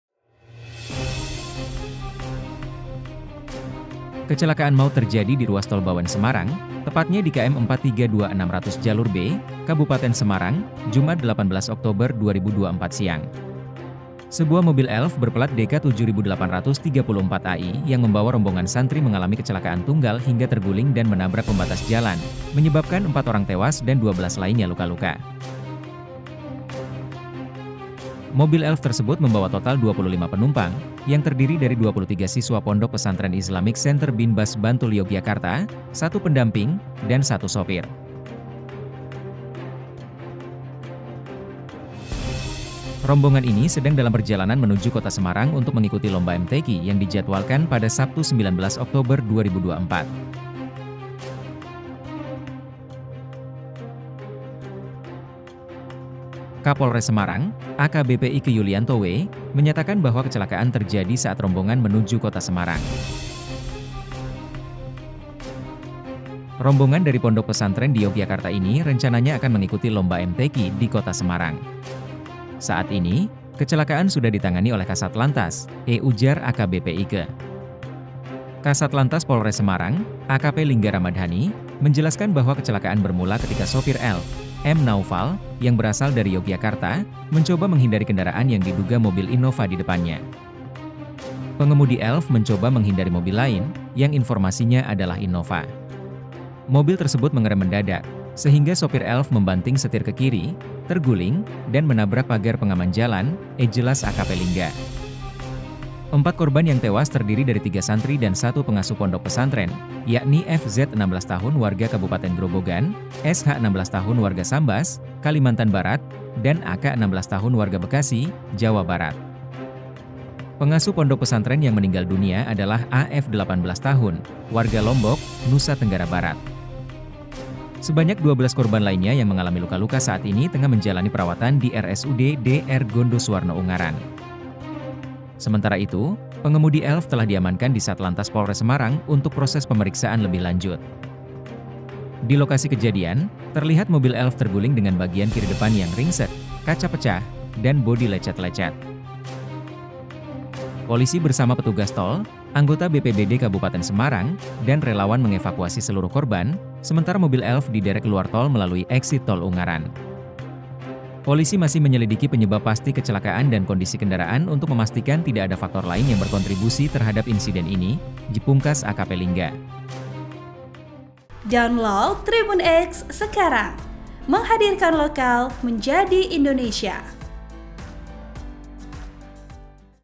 Kecelakaan Mobil Elf Rombongan Santri di Tol Bawen, 4 Orang Tewas dan 12 Luka-Luka
POS-KUPANG.COM - Kecelakaan maut terjadi di ruas Tol Bawen-Semarang, tepatnya di KM432.600 jalur B, Kabupaten Semarang, Jumat (18/10/2024) siang.

Sebuah mobil Elf berpelat DK7834AI yang membawa rombongan santri mengalami kecelakaan tunggal hingga terguling dan menabrak pembatas jalan, menyebabkan empat orang tewas dan 12 lainnya luka-luka.

Mobil Elf tersebut membawa total 25 penumpang, yang terdiri dari 23 siswa Pondok Pesantren Islamic Centre Bin Baz Bantul Yogyakarta, satu pendamping, dan satu sopir.

Rombongan ini sedang dalam perjalanan menuju Kota Semarang untuk mengikuti lomba MTQ yang dijadwalkan pada Sabtu (19/10/2024). (POS-KUPANG.COM, TribunJateng.com)

Penulis  : Reza Gustav Pradana/TribunJateng.com
Editor  : Daniel Ari Purnomo/TribunJateng.com
VPO  : Leo R